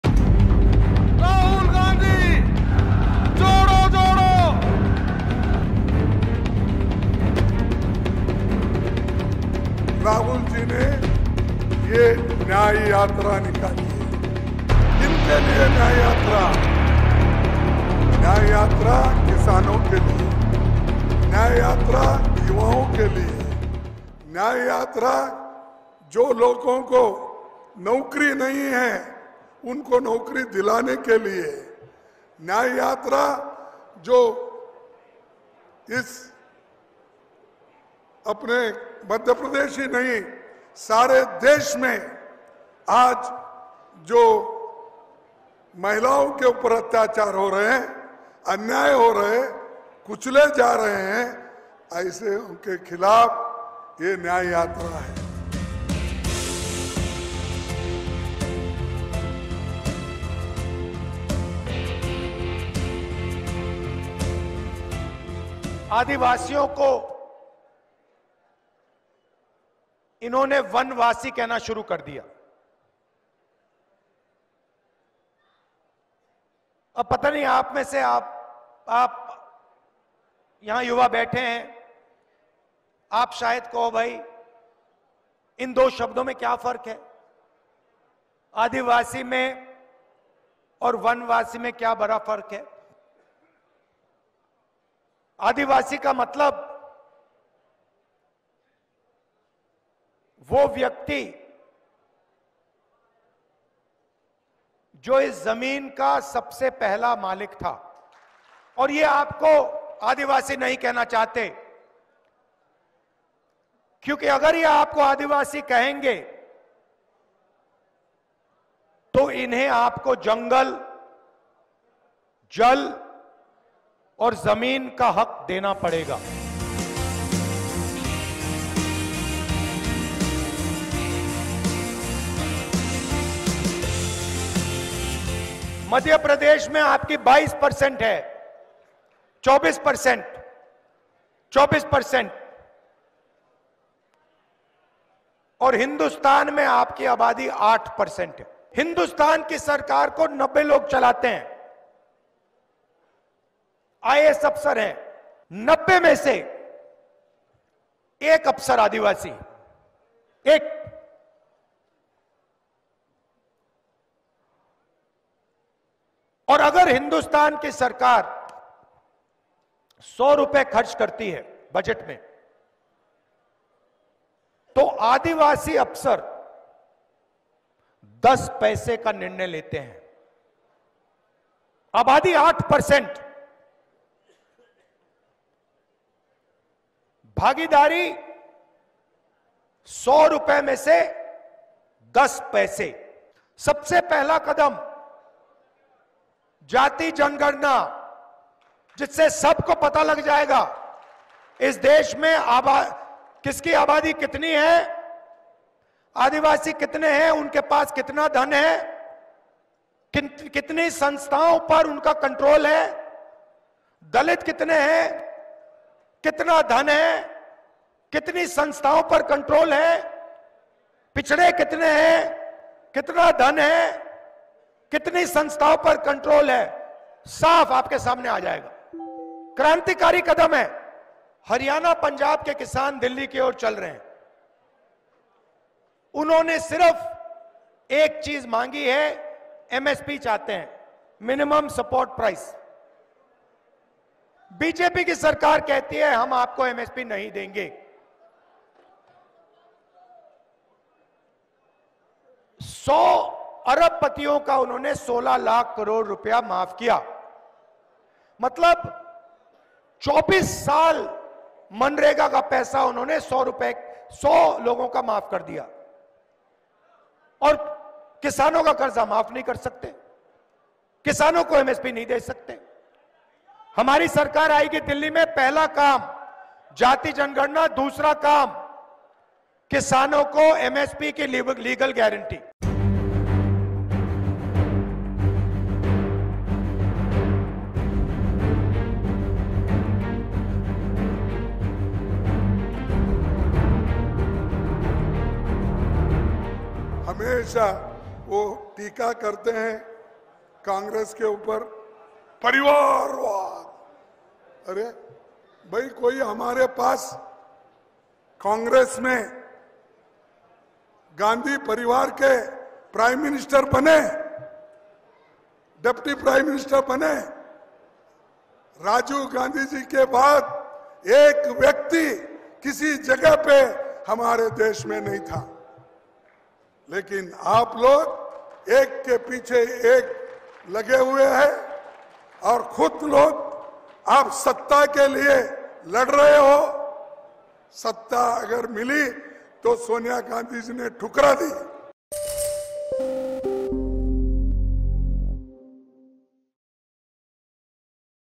Rahul Gandhi Promises MSP To Farmers If Elected To Power | N18V | CNBC-TV18
Congress leader Rahul Gandhi promises MSP for farmers if they come to power while speaking at a Bharat Jodo Nyay Yatra in Madhya Pradesh. He also promised to conduct a caste census saying it will give a clear picture of Adivasi, Dalit & backward population in India.

n18oc_politics